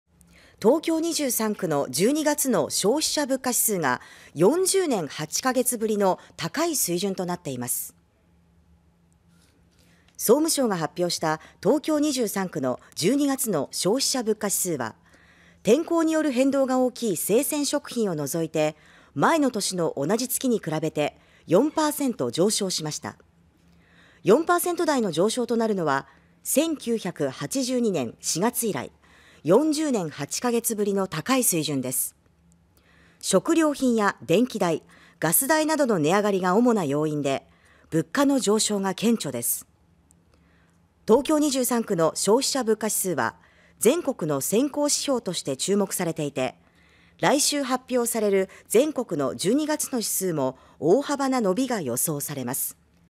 【速報】12月の東京の消費者物価4.0％上昇 40年8カ月ぶりの高水準(2023年1月10日)
東京23区の12月の消費者物価指数が40年8カ月ぶりの高い水準となっています。

　総務省が発表した東京23区の12月の消費者物価指数は天候による変動が大きい生鮮食品を除いて前の年の同じ月に比べて4％上昇しました。

　4％台の上昇となるのは1982年4月以来、40年8カ月ぶりの高い水準です。

　食料品や電気代、ガス代などの値上がりが主な要因で、物価の上昇が顕著です。

　東京23区の消費者物価指数は全国の先行指標として注目されていて、来週発表される全国の12月の指数も大幅な伸びが予想されます。